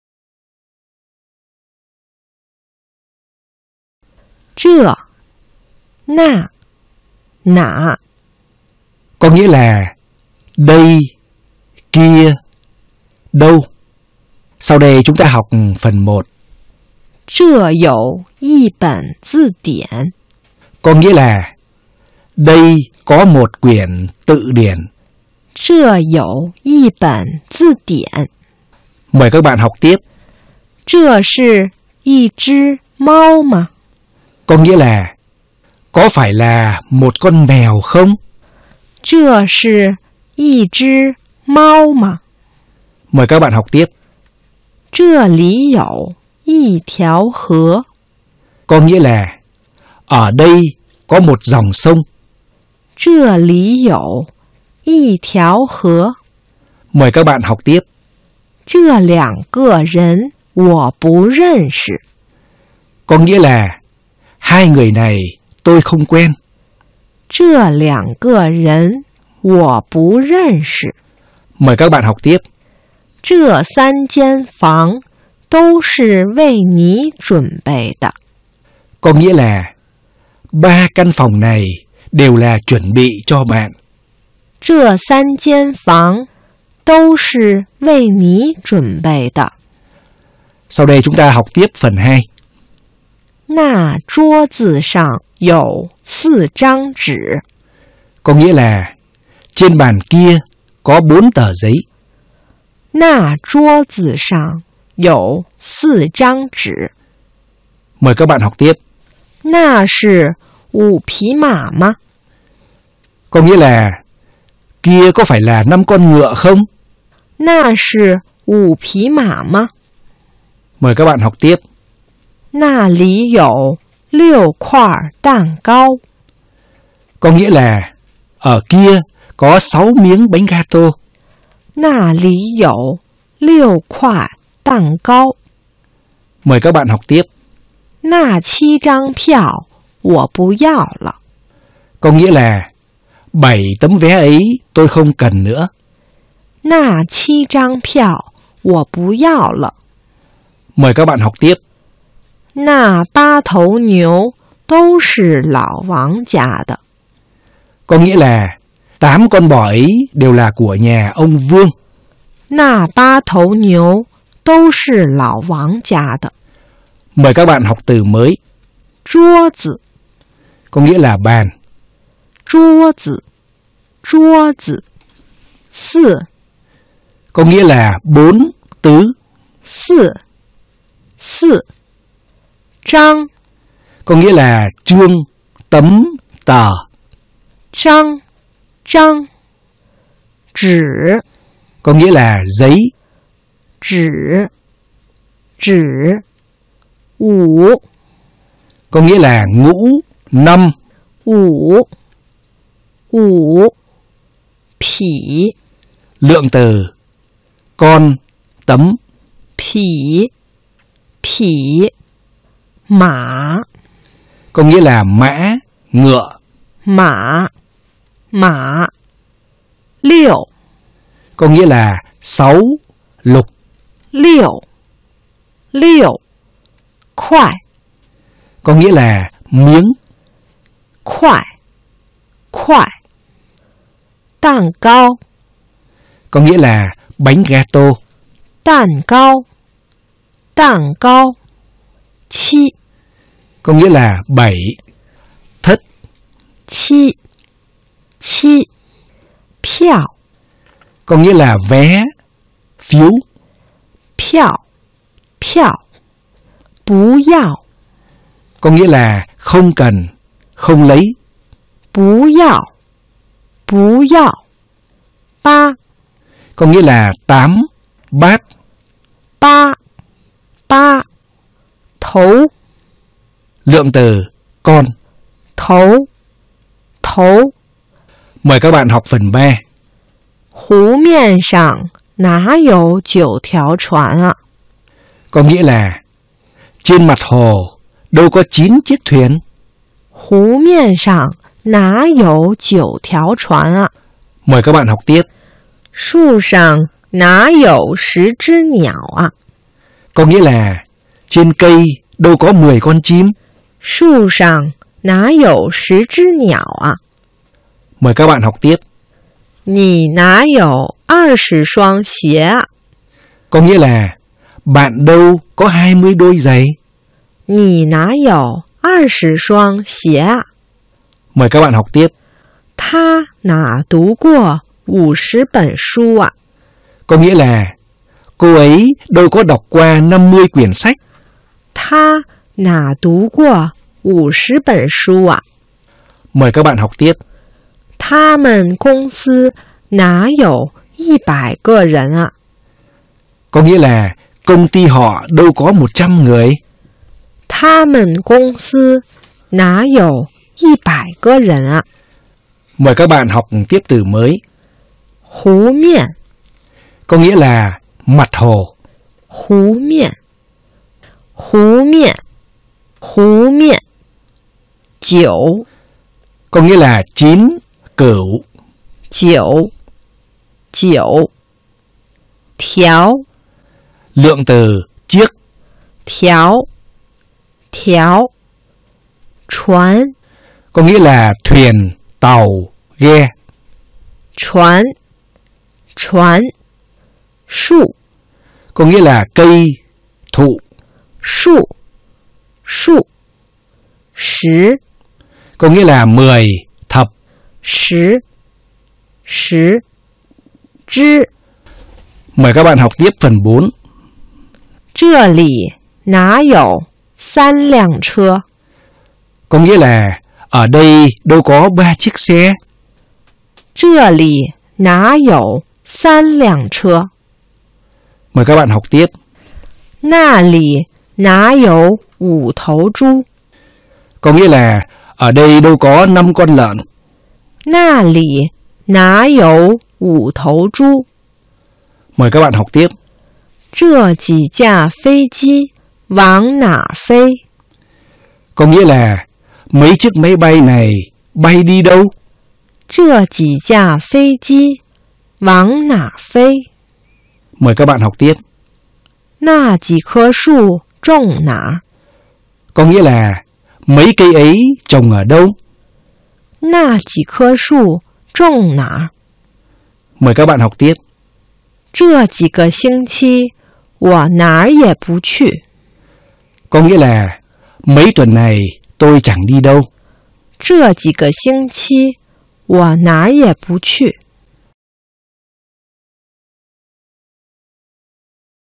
Chinh phục Tiếng Trung | "这(zhè) 那(nà) 哪(nǎ)……"句式 | 33
"这(zhè) 那(nà) 哪(nǎ)……"

Có nghĩa là "Đây/kia/đâu+số từ+lượng từ+danh từ".

下面我们来学习第一部分：

Sau đây chúng ta học phần một.

这(zhè) 有(yǒu) 一(yì) 本(běn) 字(zì) 典(diǎn)。

Đây có một quyển tự điển.

这(zhè) 是(shì) 一(yì) 只(zhī) 猫(māo) 吗(ma) ？

Đây có phải là một con mèo không ?

这(zhè) 里(lǐ) 有(yǒu) 一(yì) 条(tiáo) 河(hé) 。

Ở đây có một dòng sông.

这(zhè) 两(liǎng) 个(gè) 人(rén) 我(wǒ) 不(bú) 认(rèn) 识(shí) 。

Hai người này tôi không quen.

这(zhè) 三(sān) 间(jiān) 房(fáng) 都(dōu) 是(shì) 为(wéi) 你(nǐ) 准(zhǔn) 备(bèi) 的(de) 。

Ba căn phòng này đều là chuẩn bị cho bạn.

Mời các bạn học từ mới.

下面我们来学习第二部分：

Sau đây chúng ta học tiếp phần 2.

那(nà) 桌(zhuō) 子(zi) 上(shàng) 有(yǒu) 四(sì) 张(zhāng) 纸(zhǐ)。

Trên bàn kia có bốn tờ giấy.

那(nà) 是(shì) 五(wǔ) 匹(pǐ) 马(mǎ) 吗(ma)？

Kia có phải là năm con ngựa không ?

那(nà) 里(lǐ) 有(yǒu) 六(liù) 块(kuài) 蛋(dàn) 糕(gāo)。

Ở kia có sáu miếng bánh ga-tô.

那(nà) 七(qī) 张(zhāng) 票(piào) 我(wǒ) 不(bú) 要(yào) 了(le)。

Bảy tấm vé ấy tôi không cần nữa.

那(nà) 八(bā) 头(tóu) 牛(niú) 都(dōu) 是(shì) 老(lǎo) 王(wáng) 家(jiā) 的(de)。

Tám con bò ấy đều là của nhà ông Vương.

下面我们来学习第三部分：

Mời các bạn học phần ba.

湖(hú) 面(miàn) 上(shàng) 哪(nǎ) 有(yǒu) 九(jiǔ) 条(tiáo) 船(chuán) 啊(a) ？

Trên mặt hồ đâu có chín chiếc thuyền ?

树(shù) 上(shàng) 哪(nǎ) 有(yǒu) 十(shí) 只(zhī) 鸟(niǎo) 啊(a) ？

Trên cây đâu có mười con chim ?

你(nǐ) 哪(nǎ) 有(yǒu) 二(èr) 十(shí) 双(shuāng) 鞋(xié) 啊(a) ？

Bạn đâu có hai mươi đôi giầy

她(tā) 哪(nǎ) 读(dú) 过(guò) 五(wǔ) 十(shí) 本(běn) 书(shū) 啊(a) ？

Cô ấy đâu có đọc qua năm mươi quyển sách ?

他(tā) 们(men) 公(gōng) 司(sī) 哪(nǎ) 有(yǒu) 一(yì) 百(bǎi) 个(gè) 人(rén) 啊(a) ？

Có nghĩa là : Công ty họ đâu có một trăm người ?

Mời các bạn học tiếp từ mới.

下面我们来学习第四部分：

Mời các bạn học tiếp phần bốn.

这(zhè) 里(lǐ) 哪(nǎ) 有(yǒu) 三(sān) 辆(liàng) 车(chē) ？

Ở đây đâu có ba chiếc xe ?

那(nà) 里(lǐ) 哪(nǎ) 有(yǒu) 五(wǔ) 头(tóu) 猪(zhū) ？

Ở đó đâu có năm con lợn ?

这(zhè) 几(jǐ) 架(jià) 飞(fēi) 机(jī) 往(wǎng) 哪(nǎ) 儿(ér) 飞(fēi) ？

Mấy chiếc máy bay này bay đi đâu ?

那(nà) 几(jǐ) 棵(kē) 树(shù) 种(zhòng) 哪(nǎ) 儿(ér) ？

Mấy cây ấy trồng ở đâu ?

这(zhè) 几(jǐ) 个(gè) 星(xīng) 期(qī) 我(wǒ) 哪(nǎ) 也(yě) 不(bú) 去(qù) 。

Mấy tuần này tôi chẳng đi đâu.